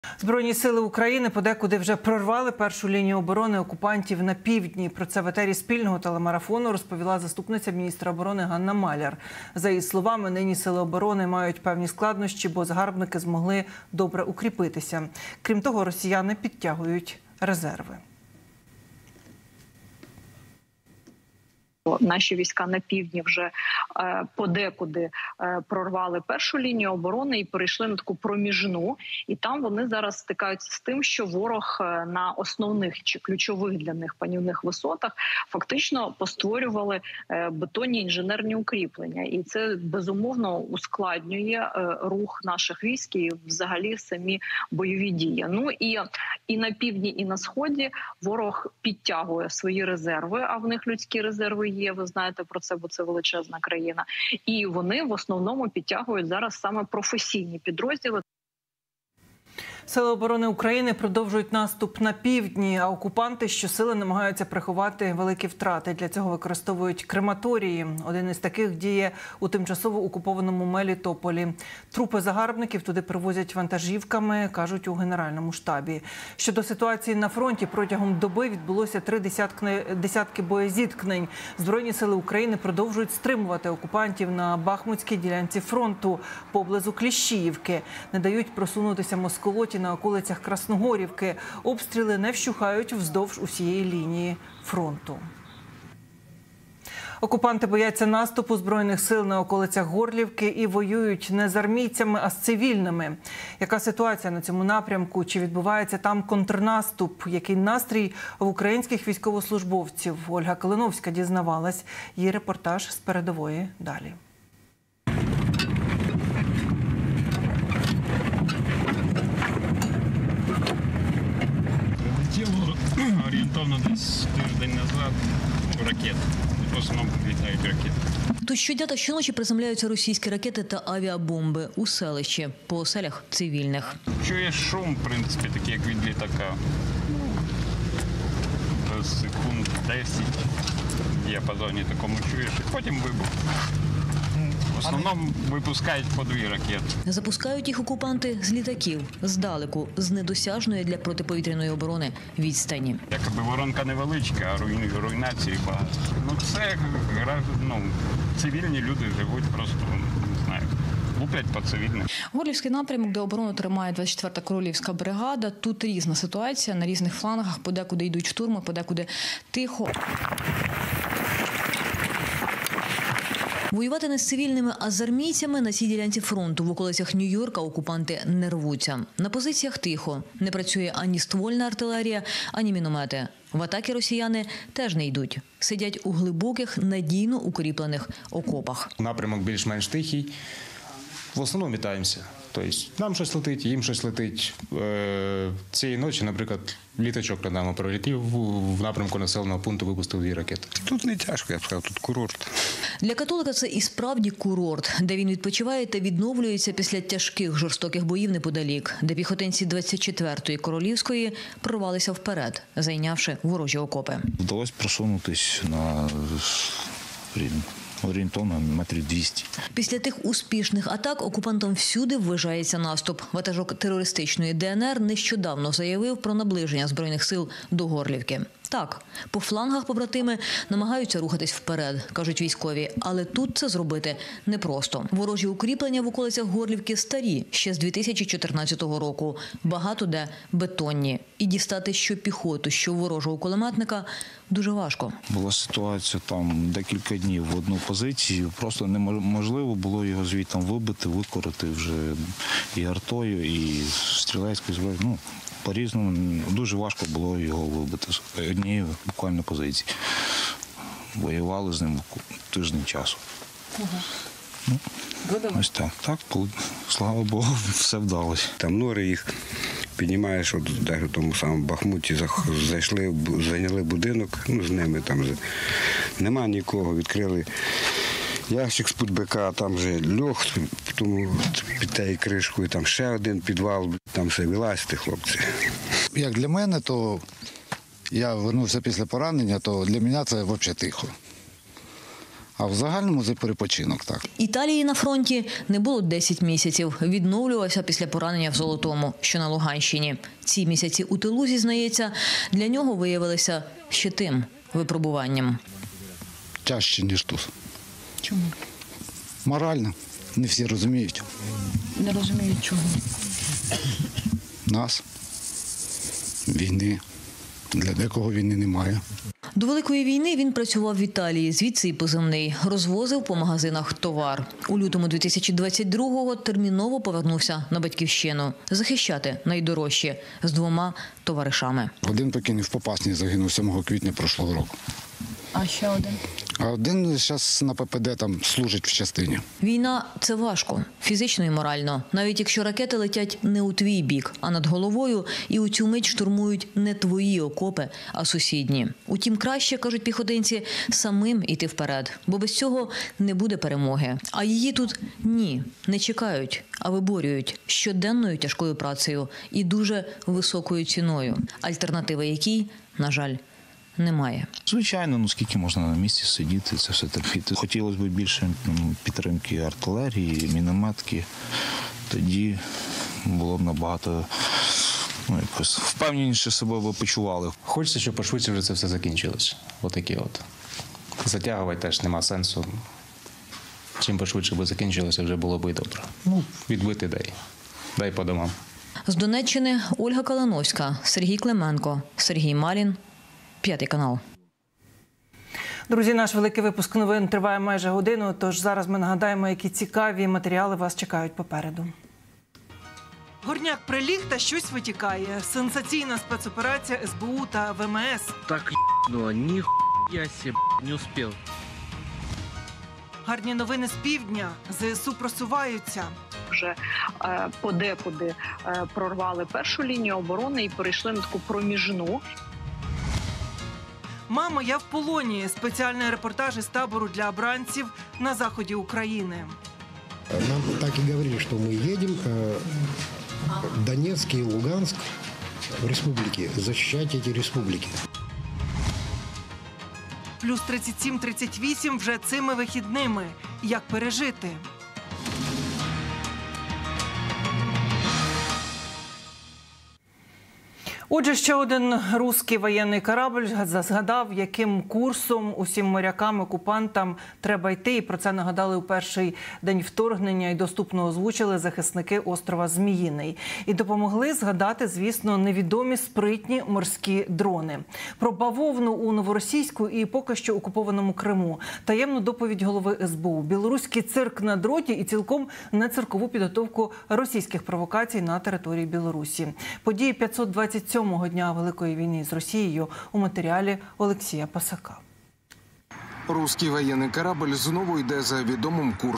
💪ЗСУ на півдні подекуди прорвали першу лінію оборони росіян – Маляр
Збройні сили України подекуди вже прорвали першу лінію оборони окупантів на півдні. Про це в етері спільного телемарафону розповіла заступниця міністра оборони Ганна Маляр. За її словами, нині сили оборони мають певні складнощі, бо загарбники змогли добре укріпитися. Крім того, росіяни підтягують резерви.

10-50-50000 - будь-яка сума має значення.

Разом переможемо!
Слава Україні!